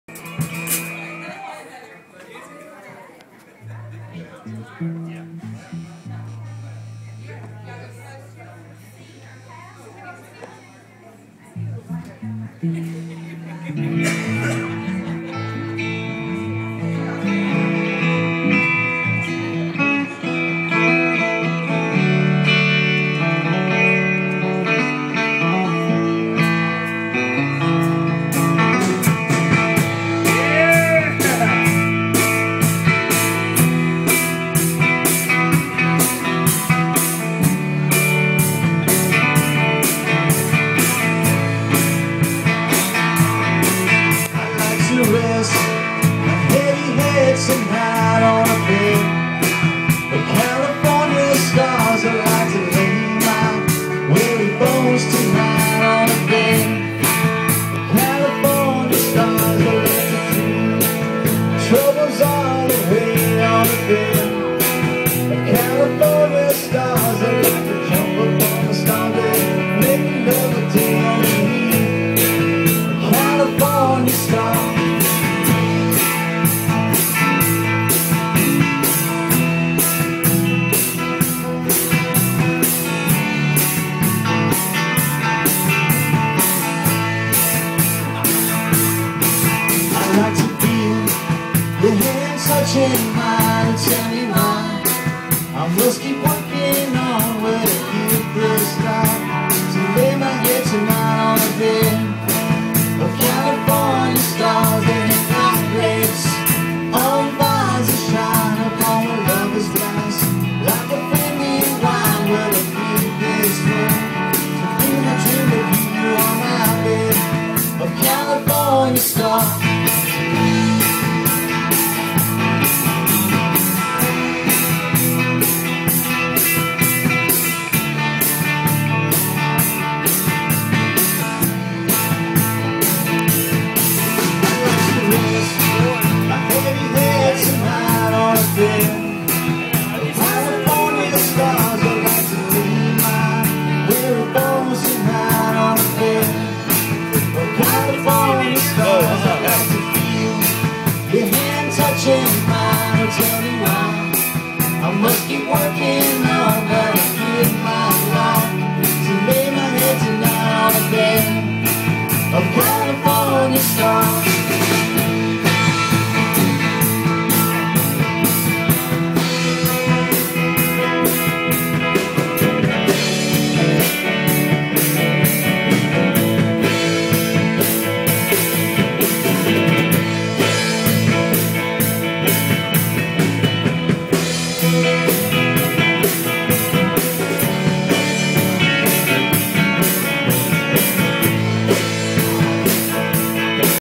Backstage Cafe Front Street
Henry's Attic concert  on the Back Deck of the Backstage Cafe on Front Street in Georgetown, SC